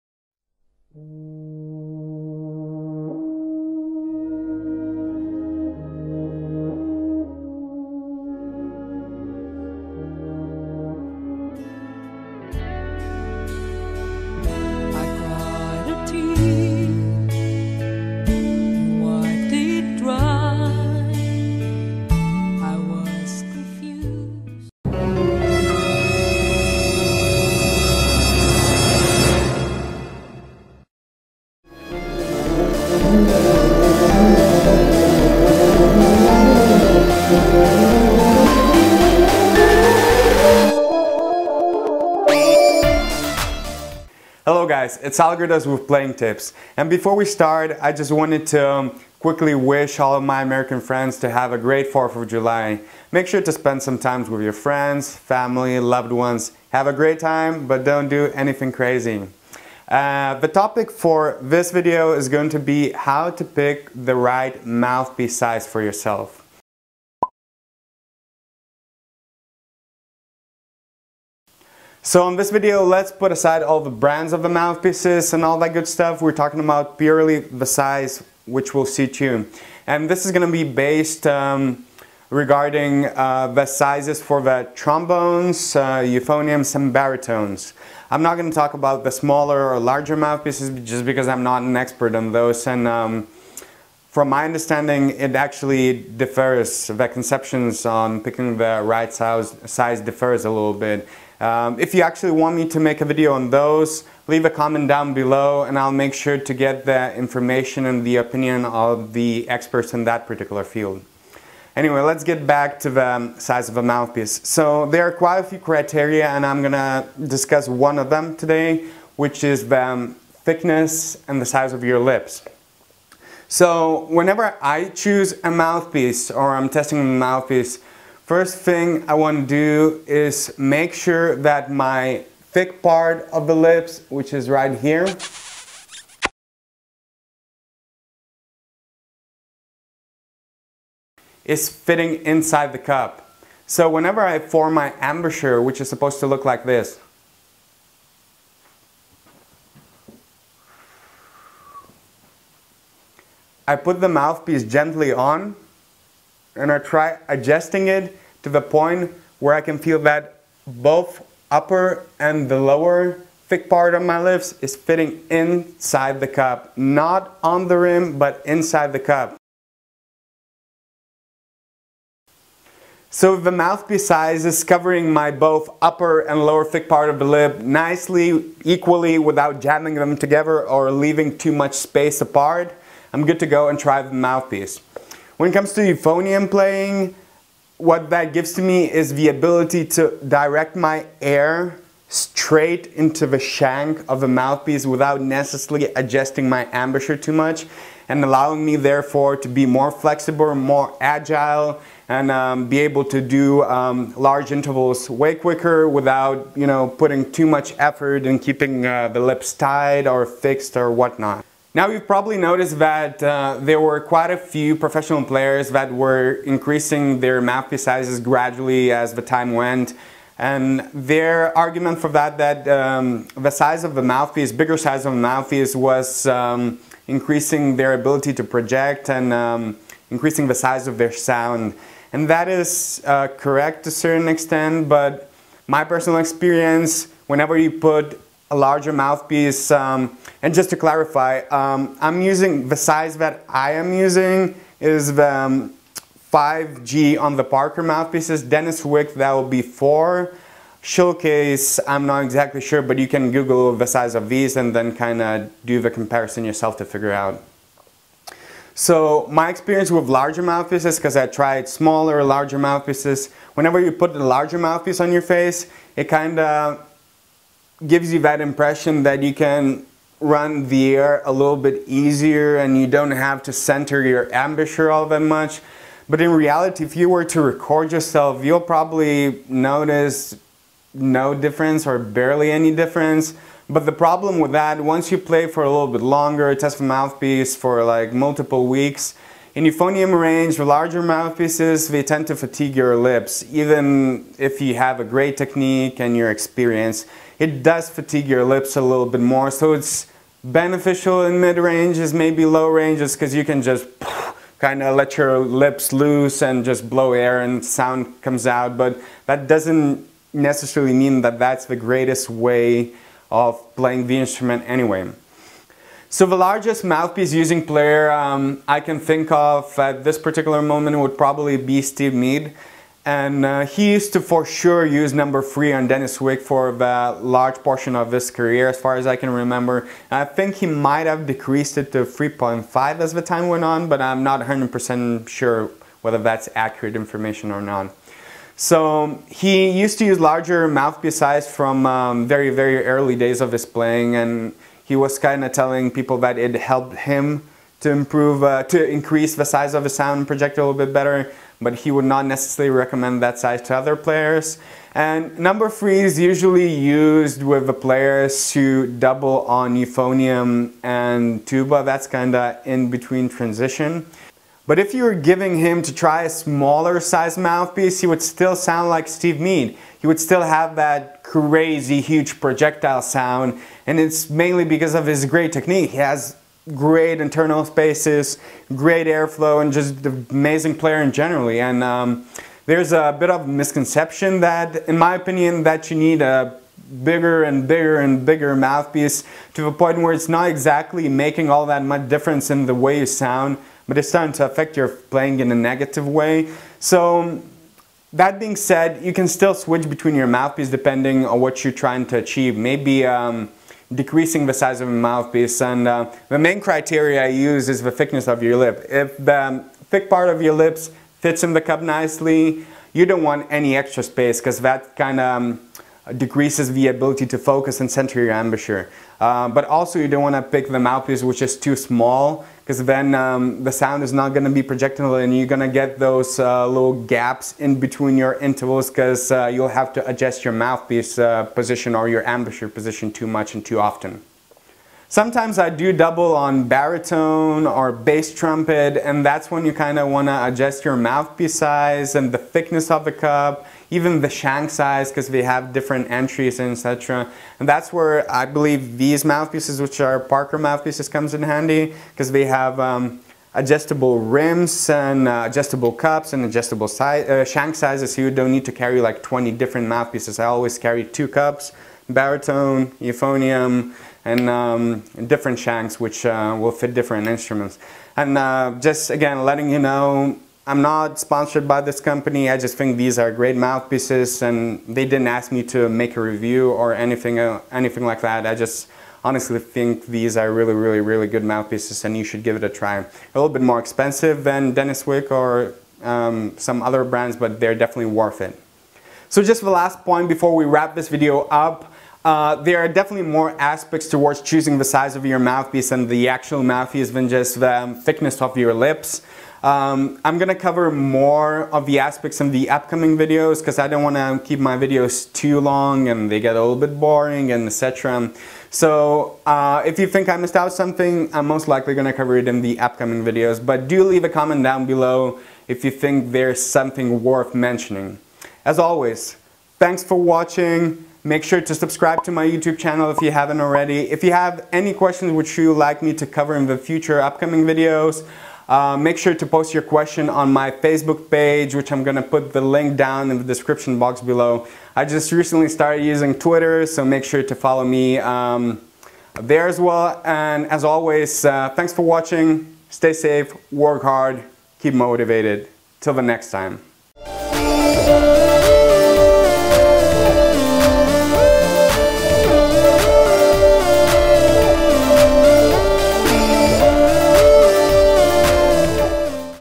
How to choose MOUTHPIECE SIZE
EUPHONIUM, TROMBONE, TUBA, TRUMPET or HORN! A quick tip which will help you determine how to pick an optimal mouthpiece size for yourself.